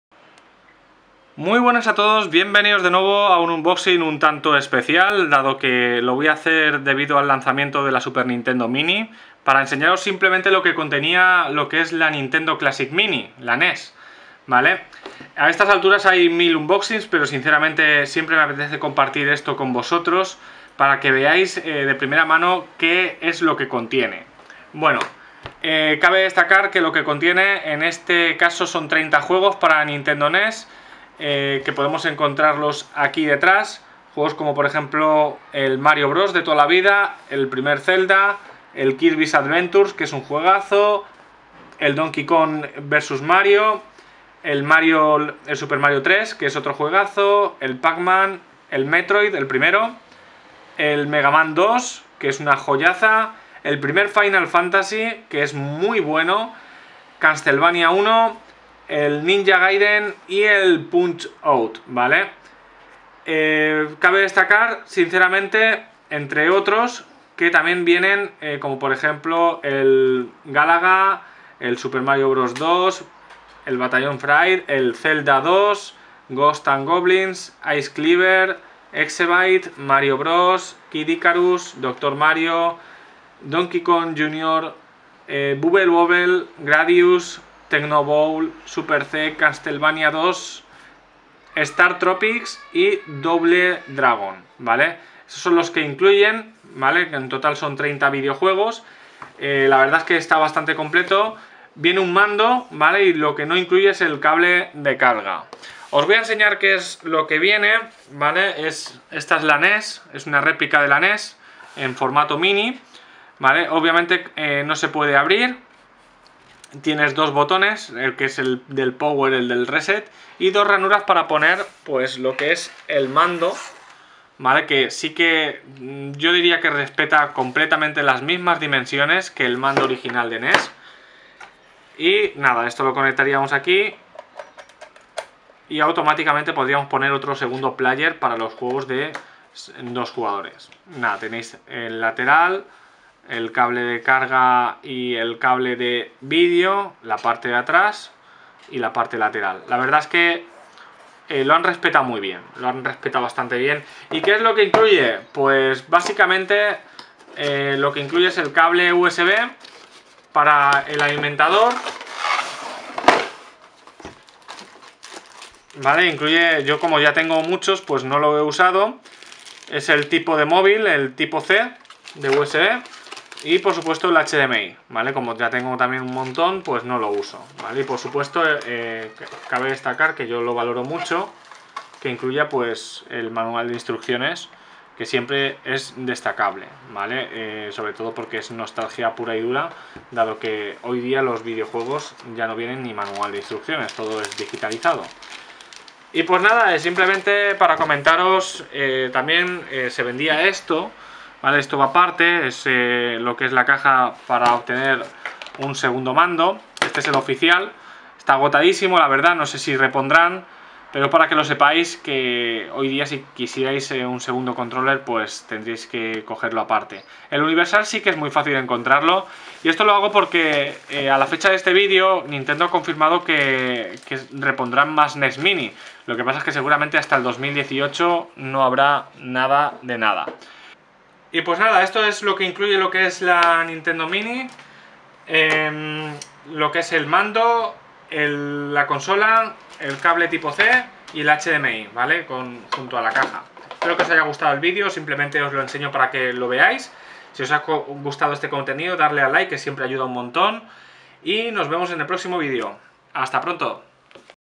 Unboxing - Nintendo Classic Mini
Aquí os dejo este unboxing

Nintendo Classic Mini: Nintendo Entertainment System
Un mando Nintendo Classic Mini: NES
Cable HDMI
Cable de alimentación USB*
30 juegos preinstalados
*Nota: para jugar con la consola se requiere un adaptador de corriente para el cable USB (no incluido).